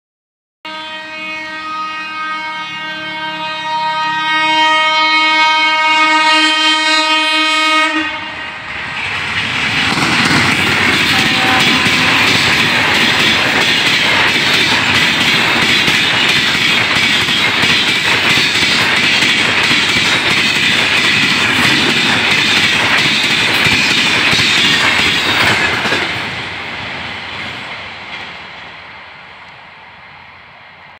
Blast At 130Kmph Of 🚉 02582/New Delhi - Manduadih SF Festival Special
نئ دِلّی - منڈوآڈيح تیوہار سپیشل     नई दिल्ली - मंडुवाडीह सुपरफास्ट त्यौहार विशेष
Max Permissible Speed: 130 km/hr between CPYZ/Chipyana Buzurg and SFG/SubedarGanj
Locomotive WAP 7 GZB